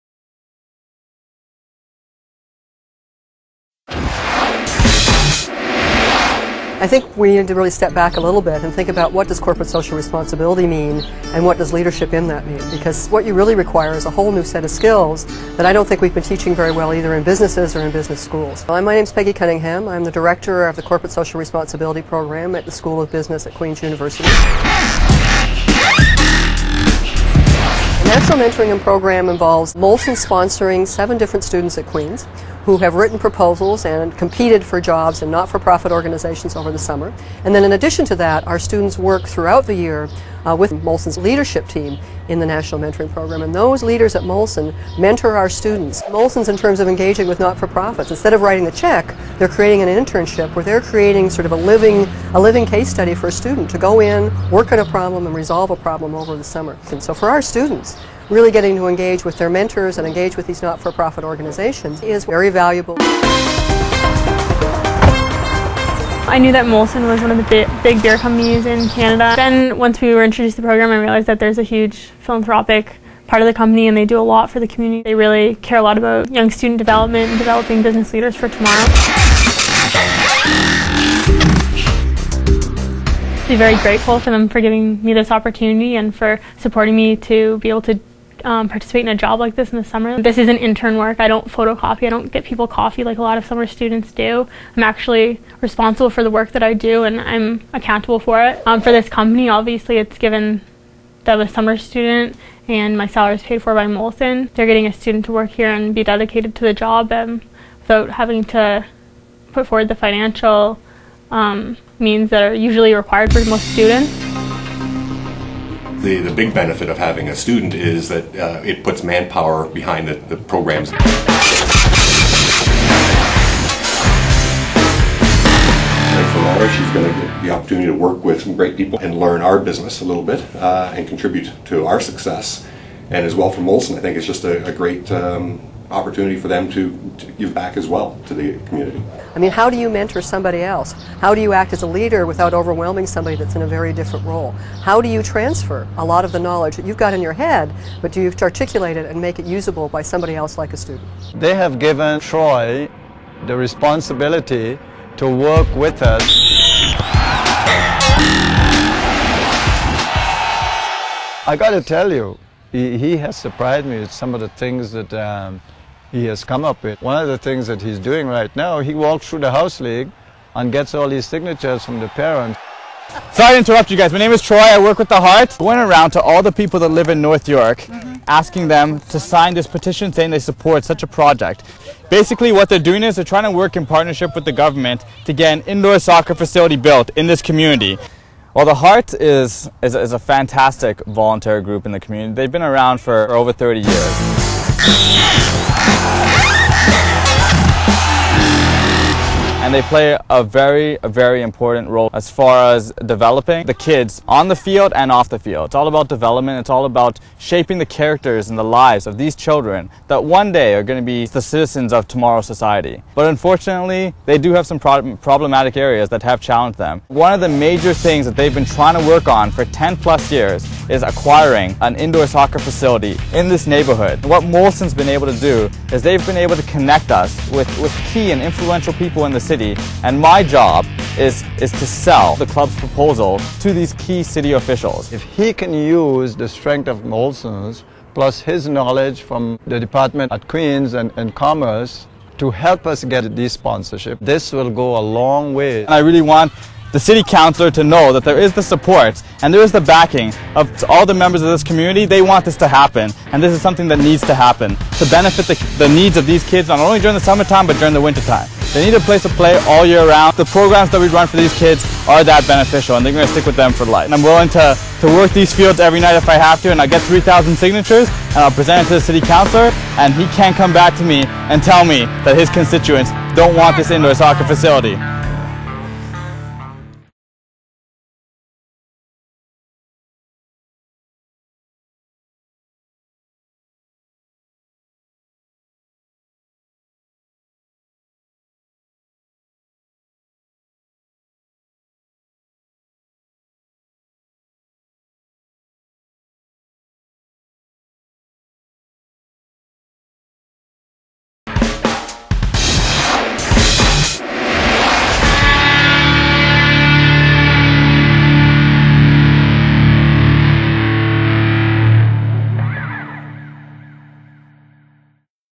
Molson VLOG Episode 8: National Mentoring Program
The National Mentoring Program connects university students with corporations. Queens University and Molson have partnered to advance the professional lives of young people.